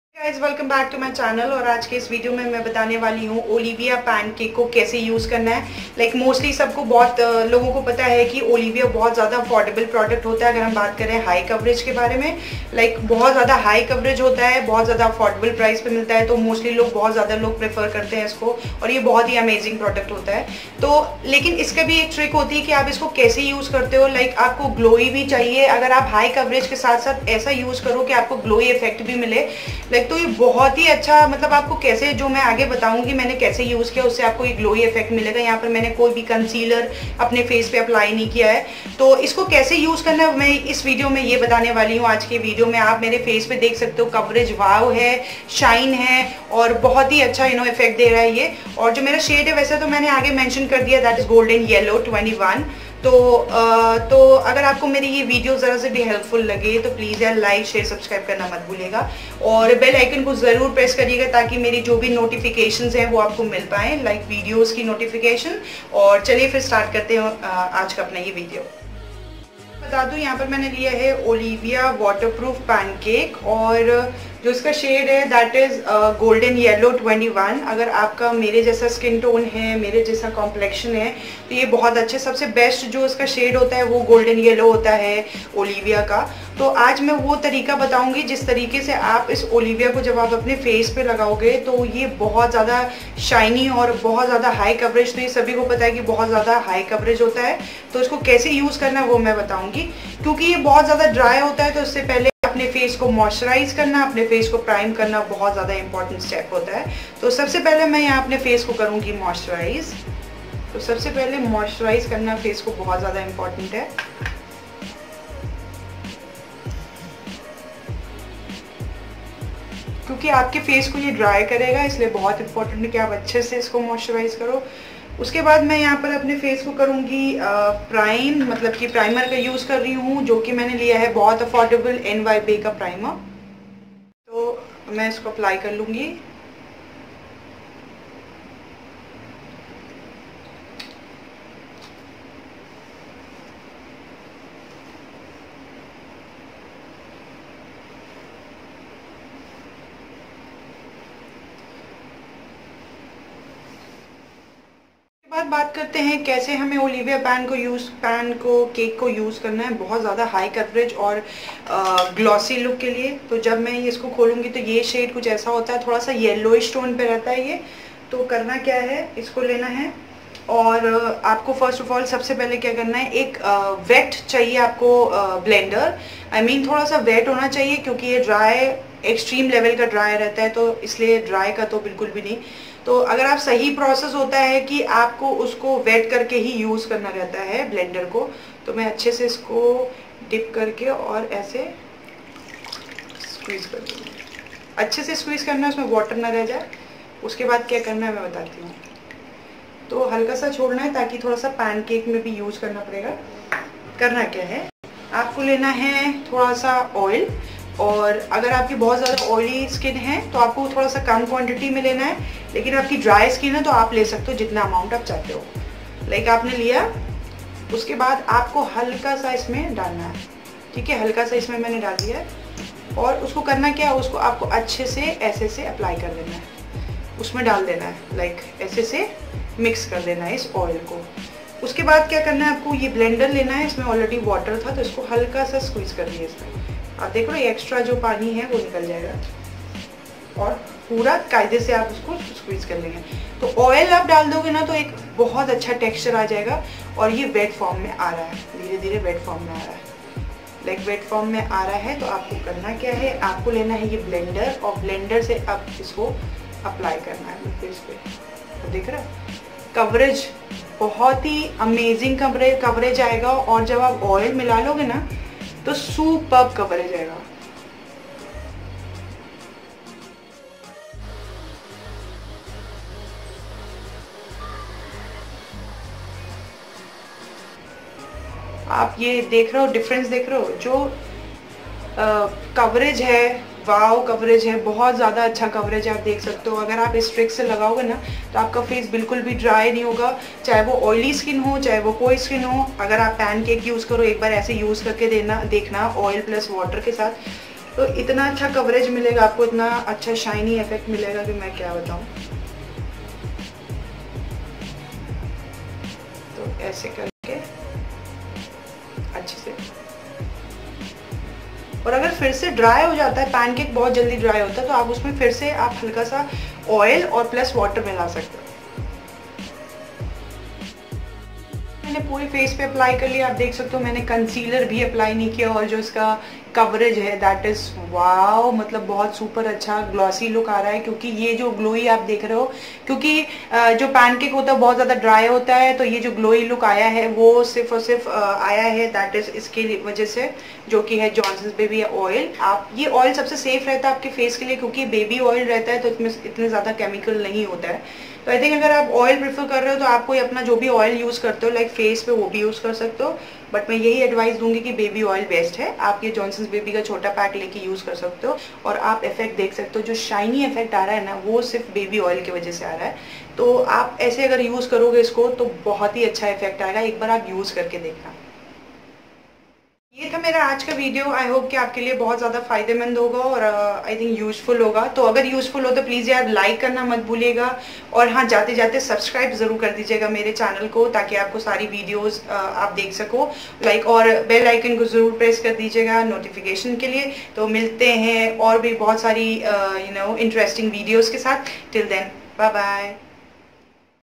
How to Apply Olivia Pan Cake to get glowy & full coverage || beauty place
How to apply Olivia Pan cake
How to use pan cake step by step
agar ap aise lagaenge pan cake to chehra aisa glow Karega aur mahenge foundation jaisa coverage milega apko ...

OLIVIA PAN CAKE
SHADE-GOLDEN YELLOW 21

Love you all bye-bye 😘😘